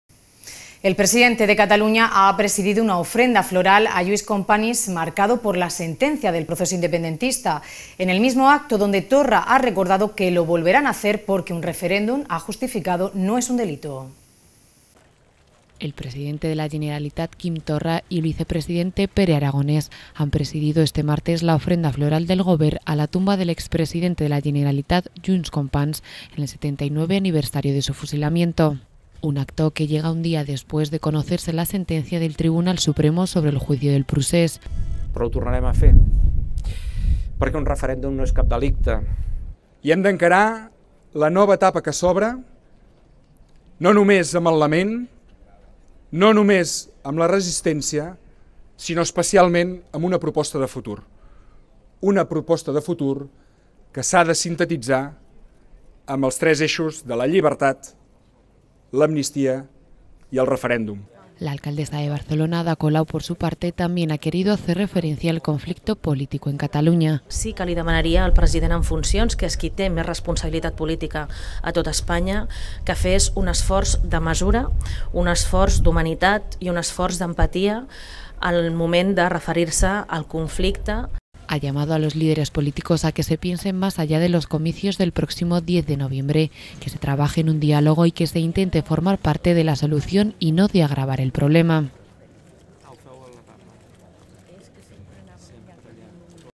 Noticia - El presidente de Cataluña ha presidido una ofrenda floral a Lluís Companys
El presidente de Cataluña ha presidido una ofrenda floral a Lluís Companys marcada por la sentencia del proceso independentista. En el mismo acto donde Torra ha recordado que lo volverán a hacer porque un referéndum, ha justificado, no es un delito.

English:

The president of Catalonia has presided over a floral offering to Lluís Companys marked by the ruling of the independence process. In the same act where Torra has remembered that they will do it again because a referendum, he has justified, is not a crime.

Live streaming